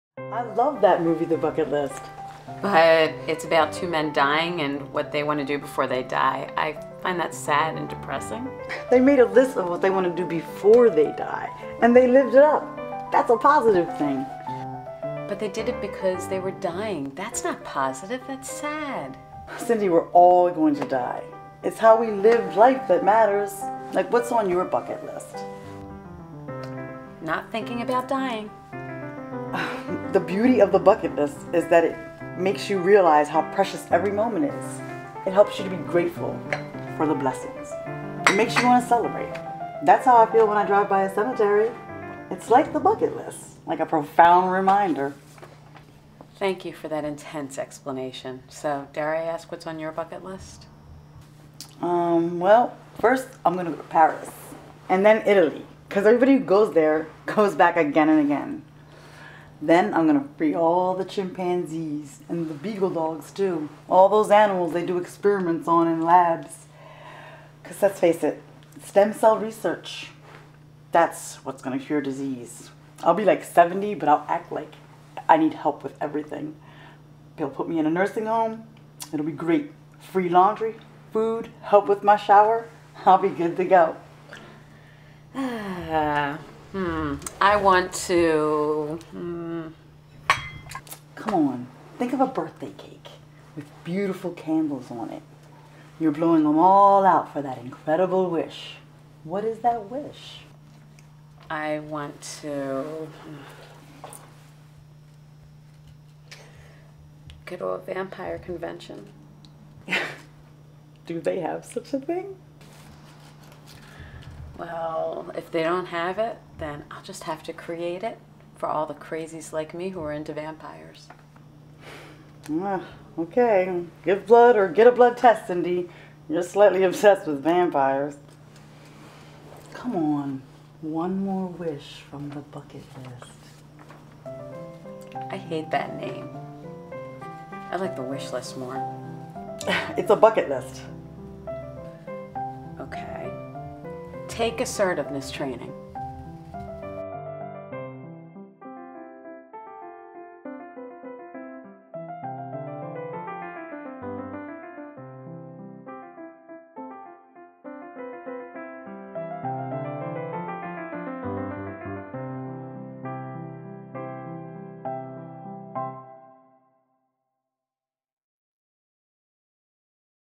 Wish
Two gals having afternoon tea.
Filmed with the Canon XH A1 with WD-H72 wide angle and 7D. Edited with Final Cut X.